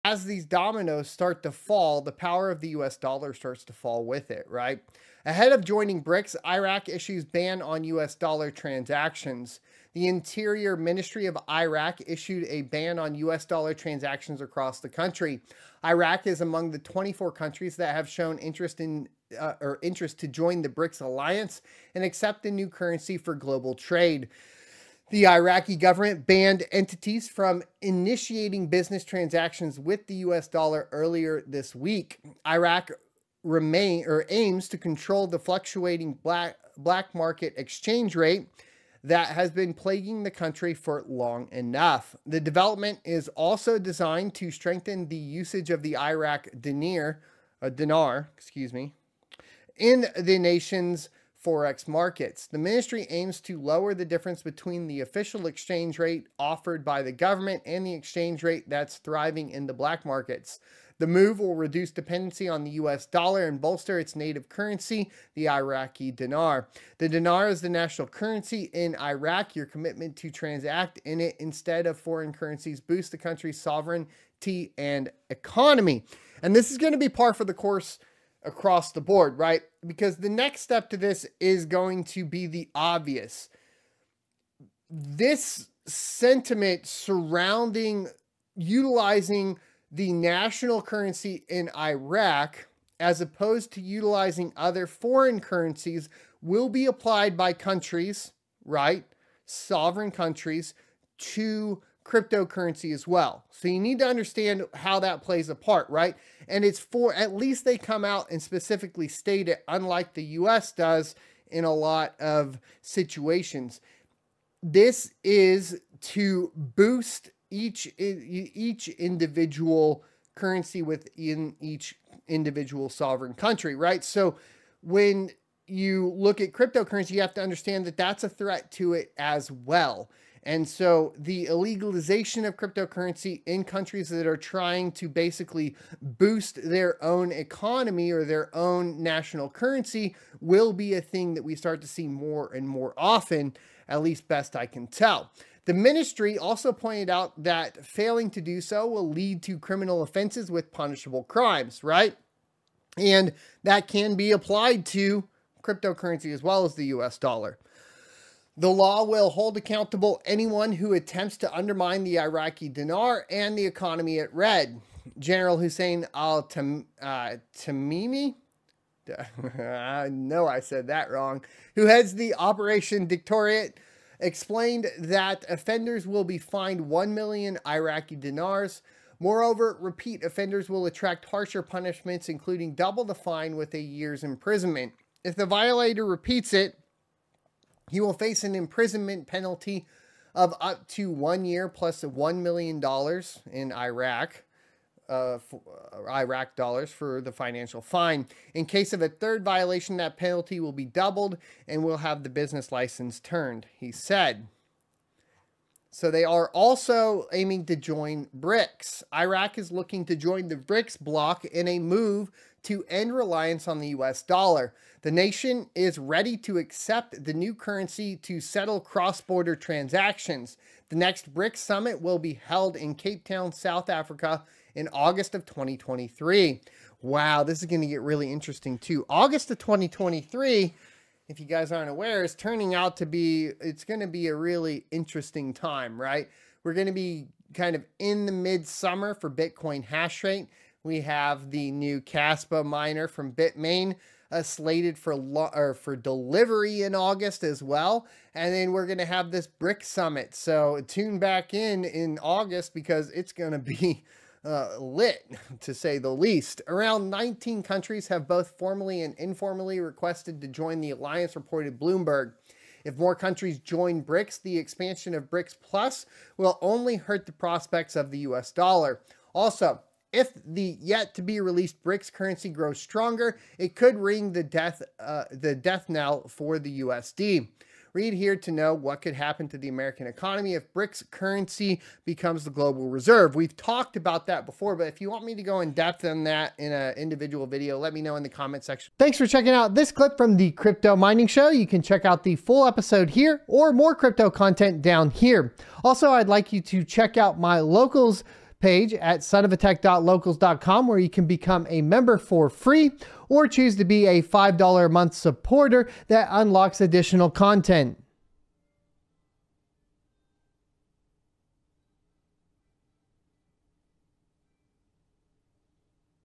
Iraq Bans U.S. Dollar Transactions - 253
Ahead of Joining BRICS, Iraq Issues Ban on U.S. Dollar Transactions

Ergo Address: 9fCyhudURbDSA2qi9k3sh1LgbHwyFwT6fck7wQW2YYWk9xR1miN
ETH Address: 0x8446b70aA05c811d7bF590e6EDE3d90C00391FcE
ETC Address: 0x8446b70aA05c811d7bF590e6EDE3d90C00391FcE
ZIL Address: zil135ajqdgf9mj6r50e8t28p98xvsss8ay7cppyx5